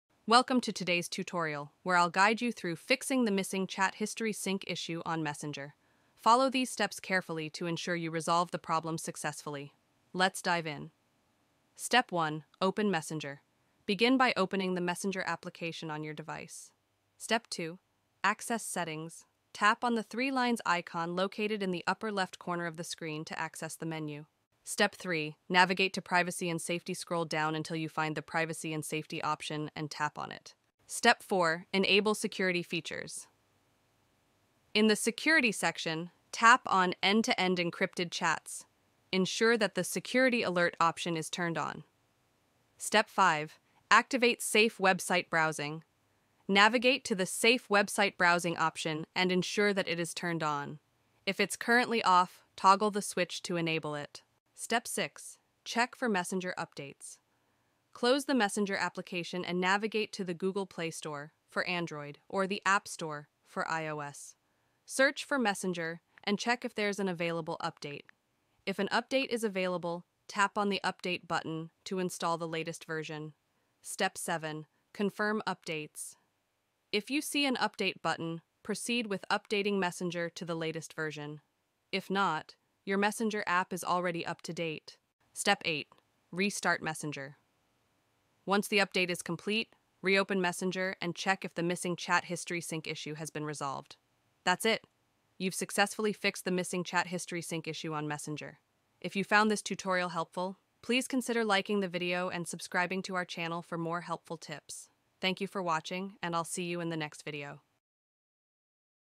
How To Fix Missing Chat History Sync Now Messenger - Remove Missing Chat History Sync Now Messenger
How To Fix Missing Chat History Sync Now Messenger - Remove Missing Chat History Sync Now On Messenger. Welcome to today's tutorial where I'll guide you through fixing the missing chat history sync issue on Messenger. Follow these steps carefully to ensure you resolve the problem successfully. Let's dive in!

Step 1: Open Messenger
Begin by opening the Messenger application on your device.

Step 2: Access Settings
Tap on the three lines icon located in the upper left corner of the screen to access the menu.

Step 3: Navigate to Privacy and Safety
Scroll down until you find the "Privacy and Safety" option and tap on it.

Step 4: Enable Security Features
In the "Security" section, tap on "End-to-End Encrypted Chats." Ensure that the "Security Alert" option is turned on.

Step 5: Activate Safe Website Browsing
Navigate to the "Safe Website Browsing" option and ensure that it is turned on. If it's currently off, toggle the switch to enable it.

Step 6: Check for Messenger Updates
Close the Messenger application and navigate to the Google Play Store (for Android) or the App Store (for iOS). Search for "Messenger" and check if there's an available update. If an update is available, tap on the "Update" button to install the latest version.

Step 7: Confirm Updates
If you see an "Update" button, proceed with updating Messenger to the latest version. If not, your Messenger app is already up to date.

Step 8: Restart Messenger
Once the update is complete, reopen Messenger and check if the missing chat history sync issue has been resolved.

That's it! You've successfully fixed the missing chat history sync issue on Messenger.